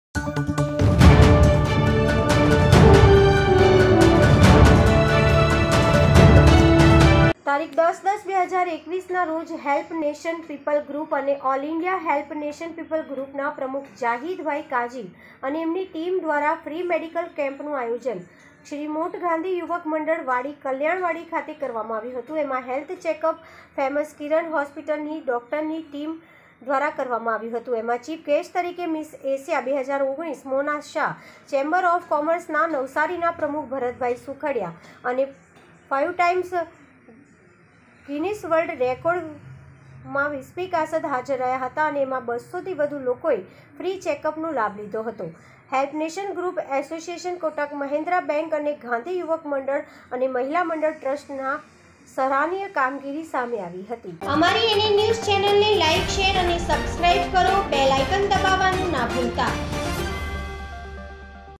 હેલ્પ નેશન પીપલ ગ્રુપ અને આેલ ઈન્ડિયા હેલ્પ નેશન પીપલ ગ્રુપ ડ્રવારા ફ્રી મેડિકલ કેમ્પન નો આયોજન
તારીખ -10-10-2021 ના રોજ *હેલ્પ નેશન પીપલ ગ્રુપ
અને આેલ ઈન્ડિયા હેલ્પ નેશન પીપલ ગ્રુપ ના પ્રમુખ ઝાહિદ ભાઈ કાઝી અને એમની ટીમ ડ્રવારા ફ્રી મેડિકલ કેમ્પન નો આયોજન  ક્ષી મોઠ ગાધીં યુવક મડળં વાડી કાલ્યાવાડી ખાતે કરવામાં આવ્યુ હતું એમા હેલ્થ ચેકઅપ  ફેમસ કિરણ હોસ્પીટલ ની ડોકટરો ટીમ ડ્રવારા કરવામાં આવ્યું હતું એમા ચિફ ગેસ્ટ તરીકે મીસ એશિયા 2019 મોના સાહ ,ચેમબર ઓફ કોમર્સના નવસારી ના પ્રમુખ ભરત  ભાઈ સુખડીયા, અને 5 ટાઈમ્સ ગીહનીસ વલ્દ રેકોર્ડ વિસ્પી કાસદ હાજર રહા હતા ,અને એમા 200 વધુ લોકો એ ફ્રી ચેકઅપ ના લાભ લીધો હતો  અને હેલ્પ નેસન ગ્રુપ અસોસીયત કોટાક મહિન્દ્રા બેંક અને ગાંધી યુવક મડળ અને મહિલા મડળ ટ્રસ્ટ  ખૂબ સરાહનીય કામગીરી કરી હતી.

ઈમરાન પઠાણ